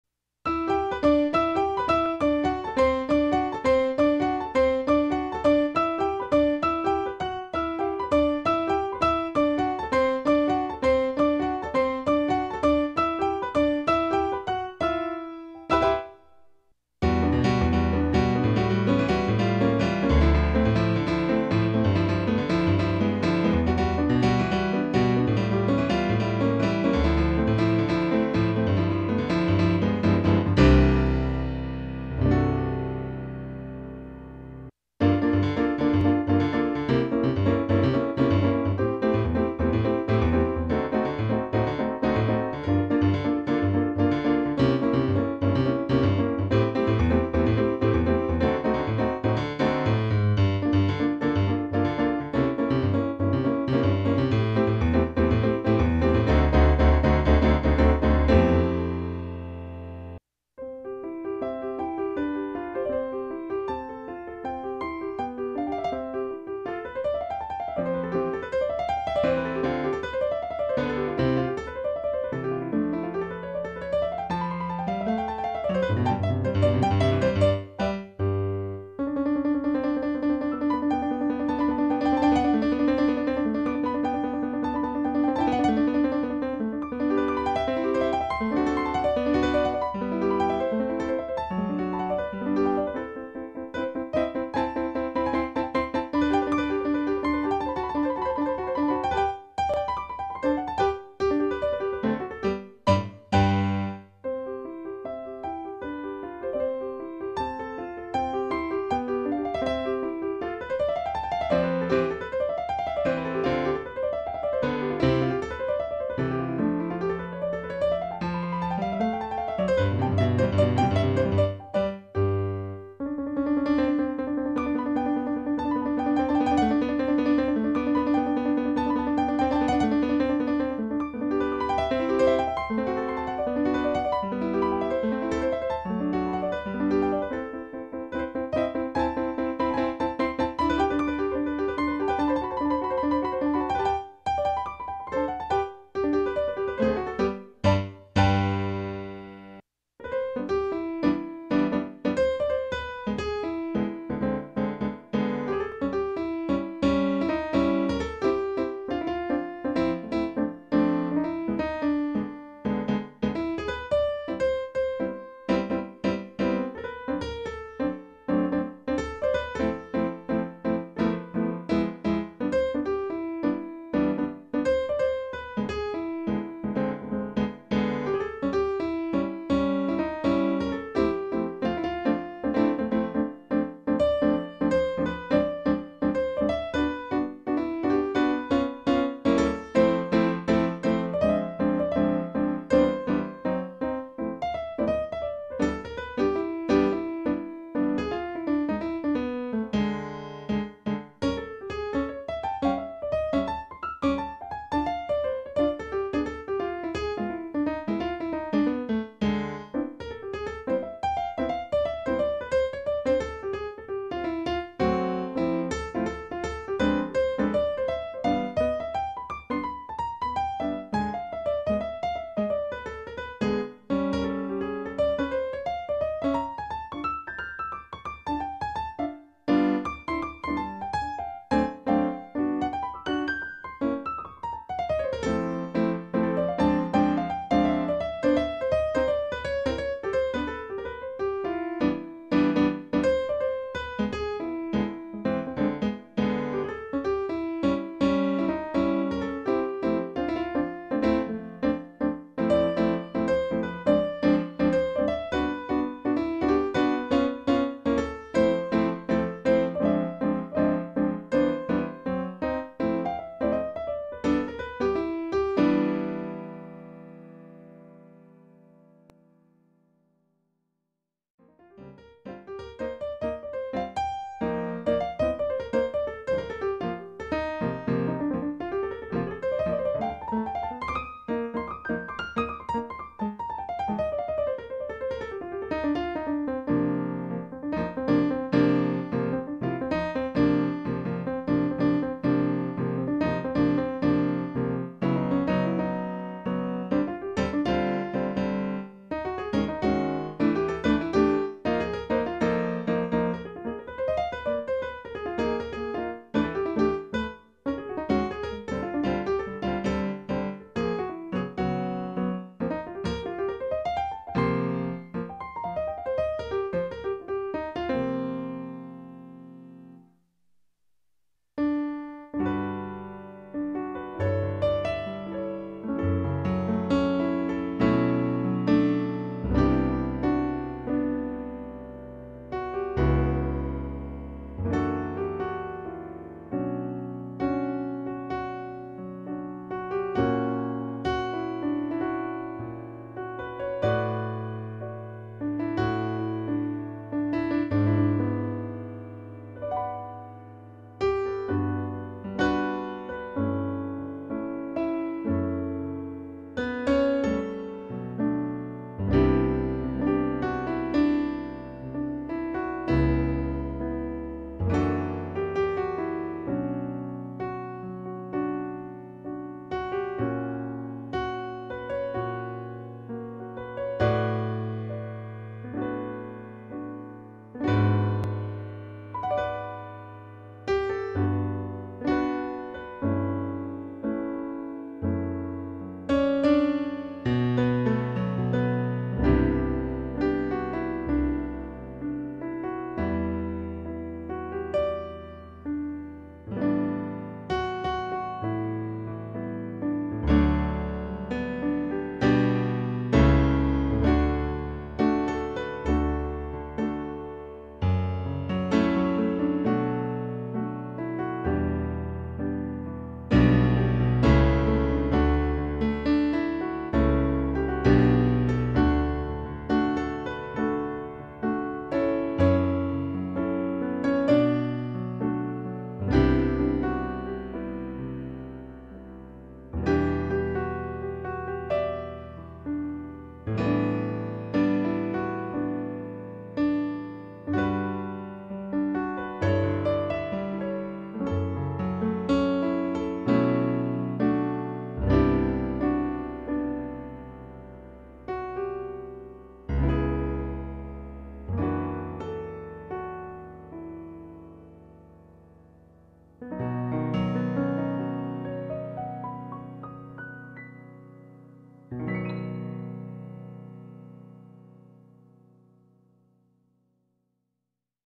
LITTLE DEMO PIANO PERFORMANCE GIOVANNI REYES
It is a little sample of proposals for working with both hands for different styles of music.